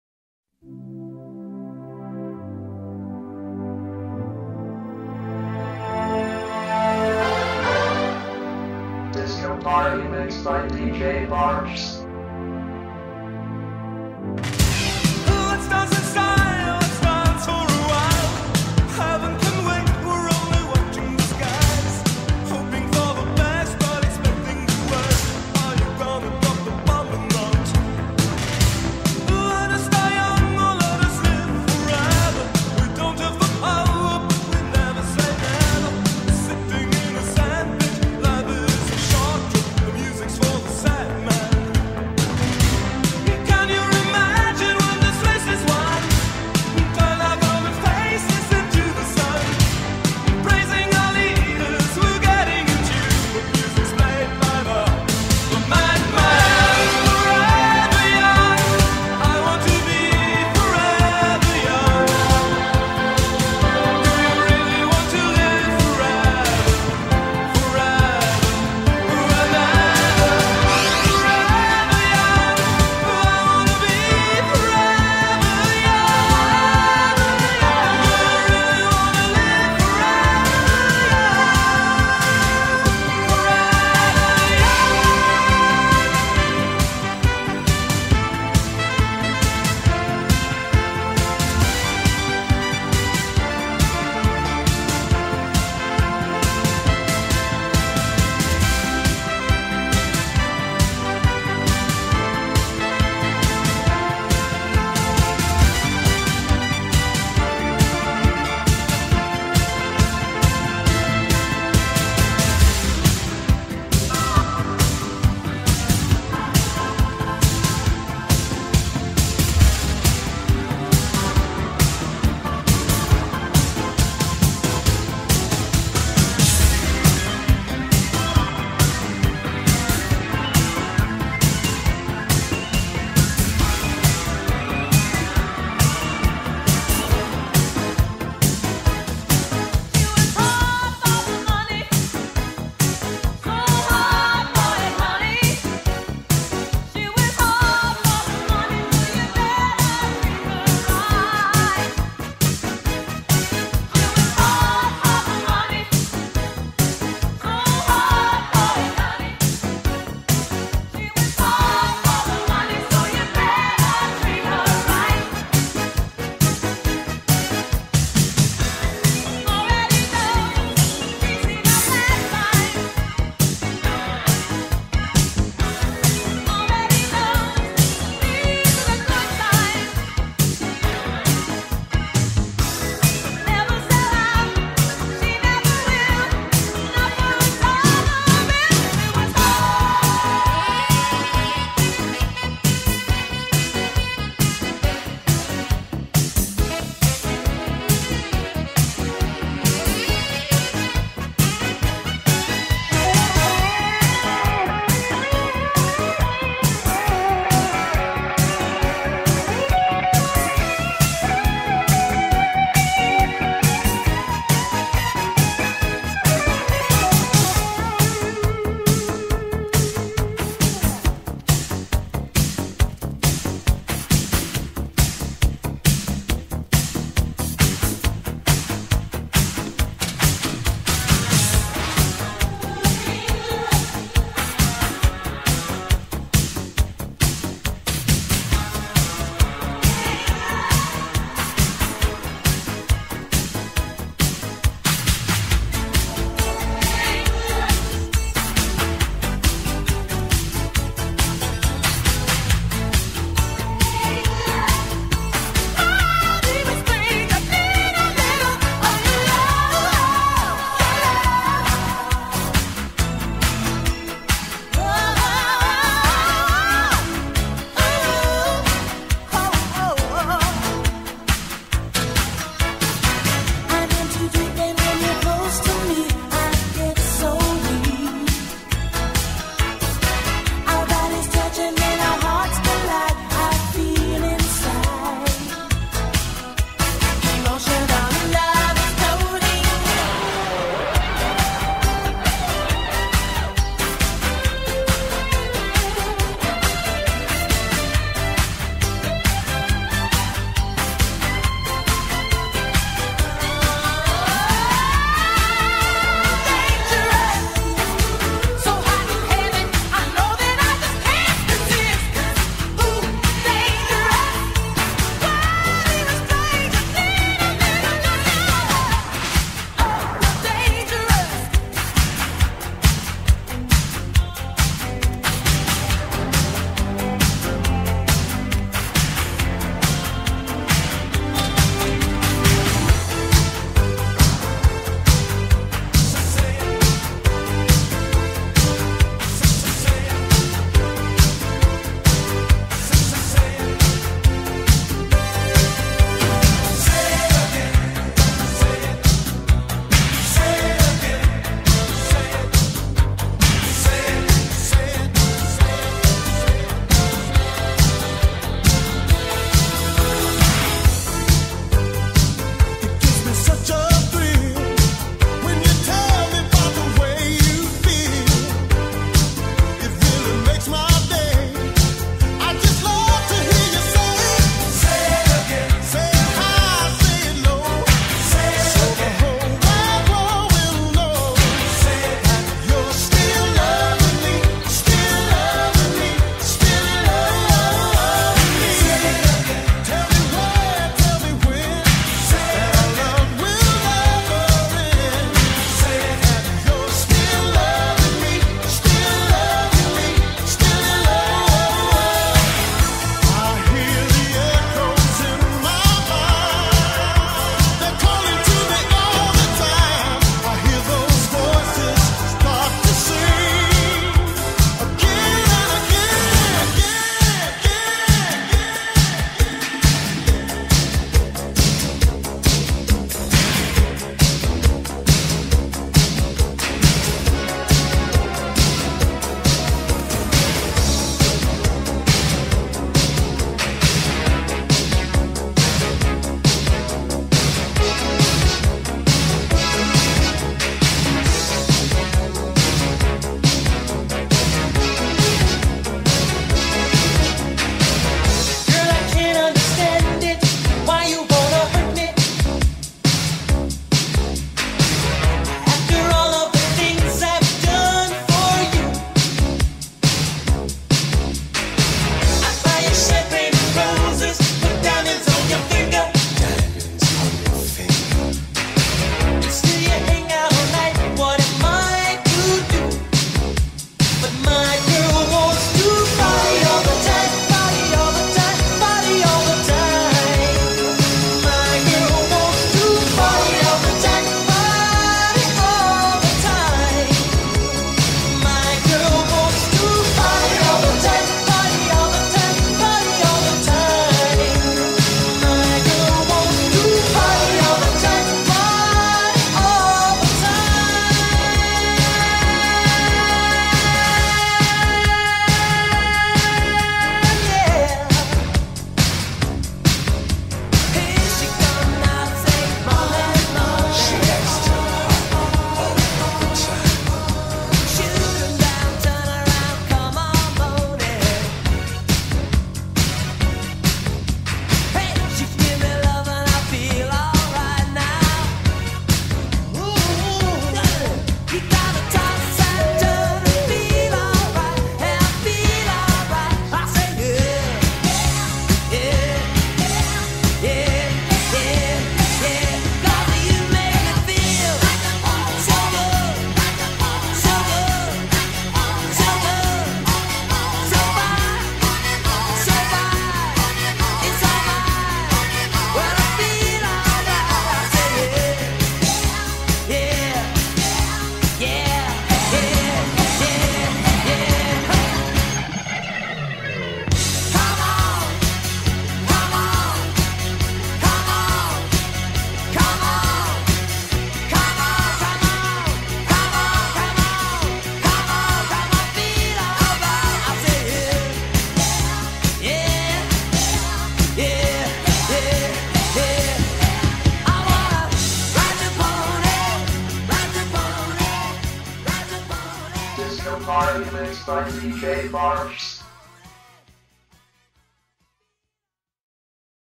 DISCO PARTY MIX 80's -=[ MIXED BY DJBARCHS ]=-
ESTA VEZ UN CONJUNTO DE TEMAS CON RITMO MÁS QUE PEGAJOSO, HACÍA UN TIEMPO QUE LOS TENÍA EN LA MIRA PORQUE SON UNOS EXCELENTES TEMAS. AGRADEZCO A LOS QUE HAN SEGUIDO MIS APORTES, SALUDOS Y DISFRUTEN.

This time a collection of more than catchy themes, it been a long time I had these on sight because they are excelents songs. Thank to people who has followed my contributions, greetings and enjoy.